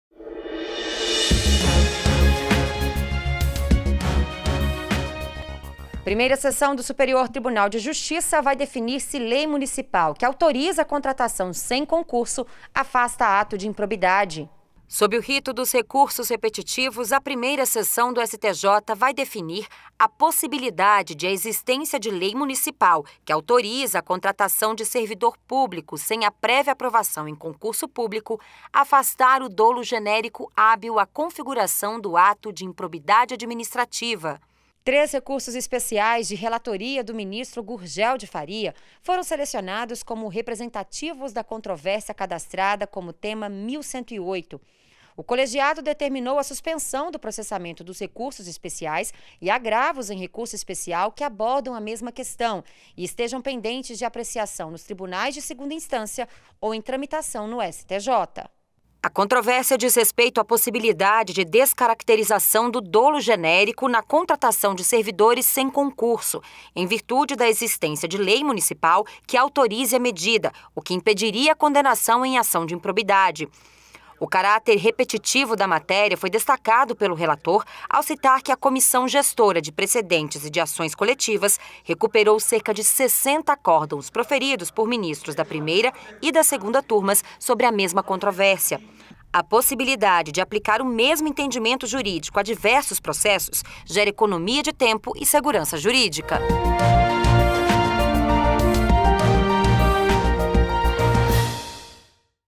Seção vai definir se lei municipal que autoriza contratação sem concurso afasta ato de improbidade
​Sob o rito dos recursos repetitivos, a Primeira Seção do Superior Tribunal de Justiça (STJ) vai definir a "possibilidade de a existência de lei municipal que autoriza a contratação de servidor público sem a prévia aprovação em concurso público afastar o dolo genérico hábil à configuração do ato de improbidade administrativa".

Foram selecionados três recursos especiais como representativos da controvérsia, cadastrada como Tema 1.108: REsp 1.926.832, REsp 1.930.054 e REsp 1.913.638. A relatoria é do ministro Gurgel de Faria.
REsp 1926832
REsp 1930054
REsp 1913638